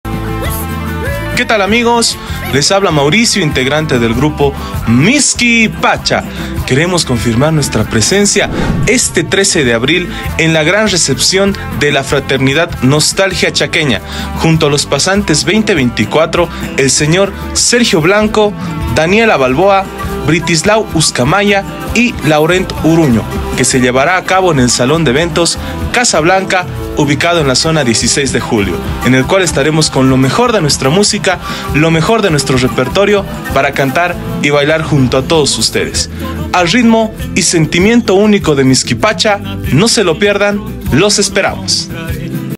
Misky Pacha estará en la Recepción Social de la Fraternidad Nostalgia Chaqueña
La Fraternidad "Nostalgia Chaqueña" Gestión 2024,
tendra a Misky Pacha en la Recepción Social que se realizará el 13 de Abril en El Alto. En honor a la Virgen del Carmen